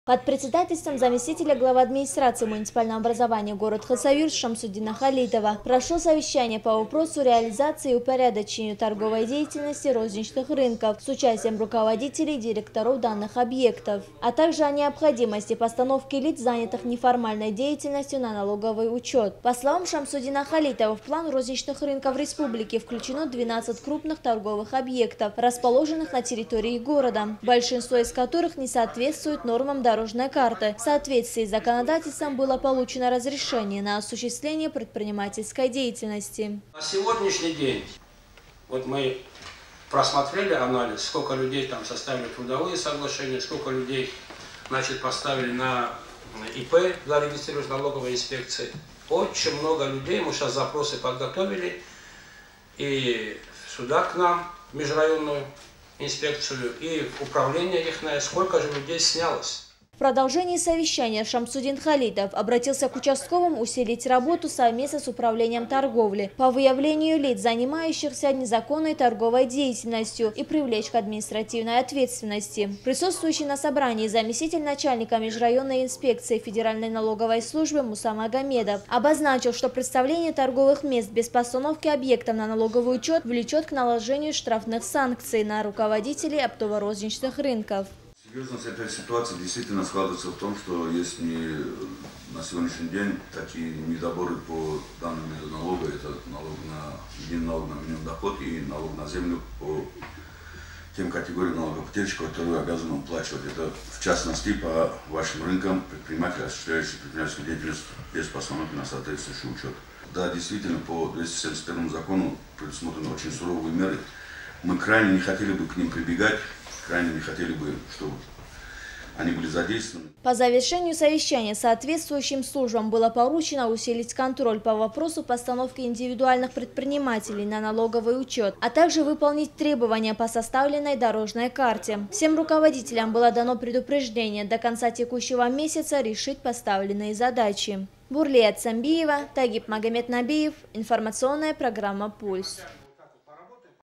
С руководителями рынков Хасавюрта обсудили проблемы предпринимательской деятельности
В администрации Хасавюрта состоялось совещание с руководителями рынков города, где обсуждению подлежала тема о необходимости постановки лиц занятых неформальной деятельностью на налоговый учет. В соответствии с законодательством было